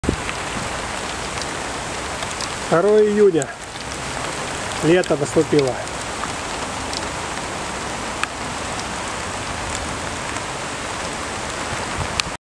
Снег в Москве в июне 2017 г. H55R4645
Продолжение пляжного сезона в июне 2017года.
Летние кафе и парки без посетителей